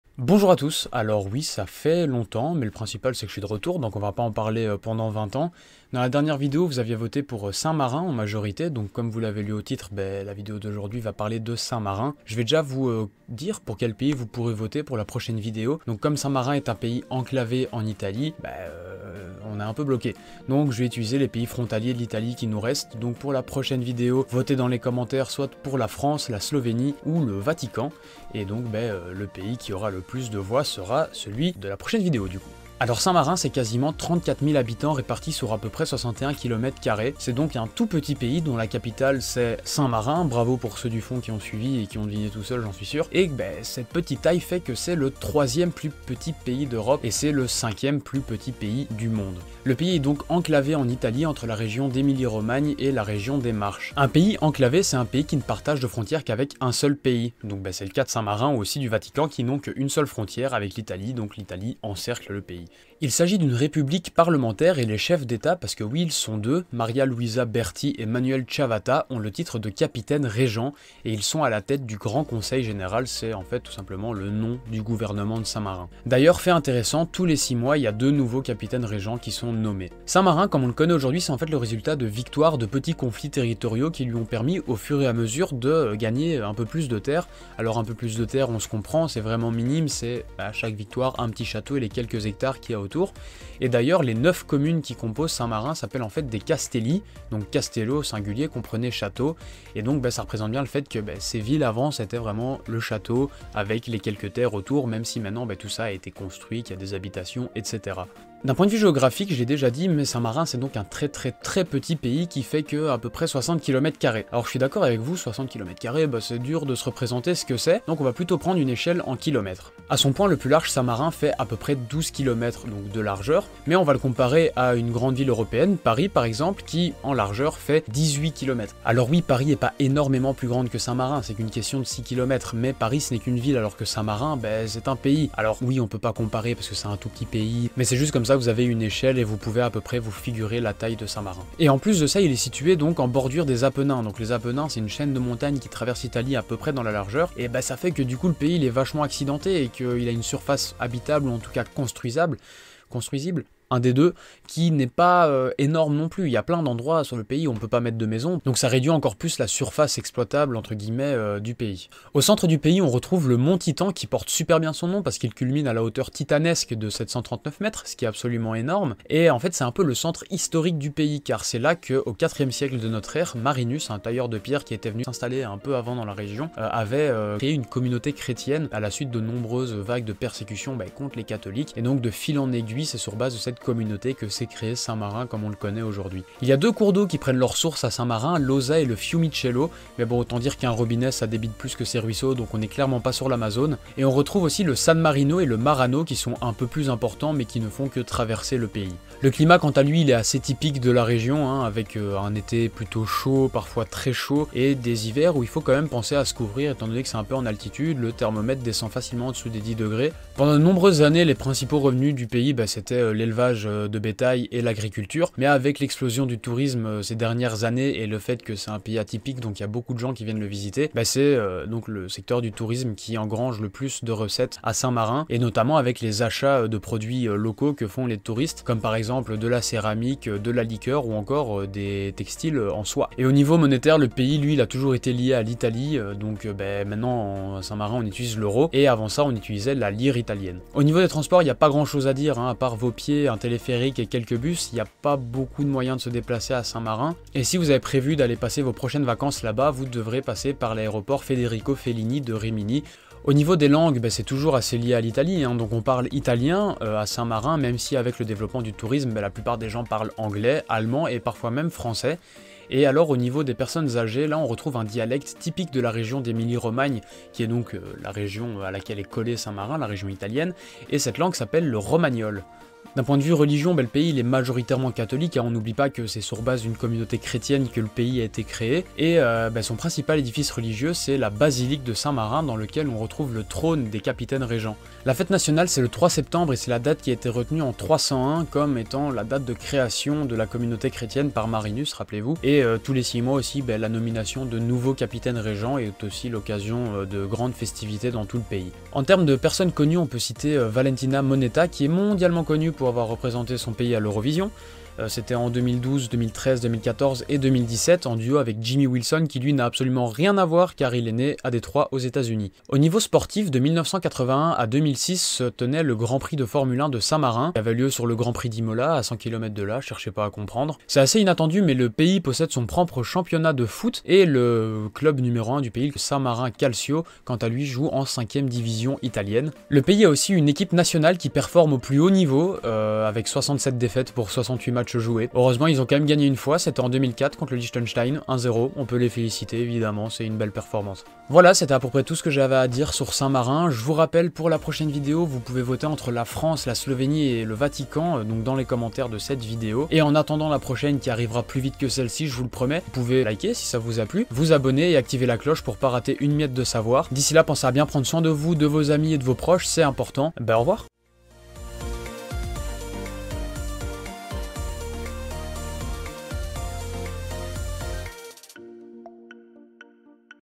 En moins de 5 minutes - Saint-Marin
Après presque deux ans d'escale en Italie, on reprend pour de bon aujourd'hui notre petit tour des pays en commençant à Saint-Marin.

Bisous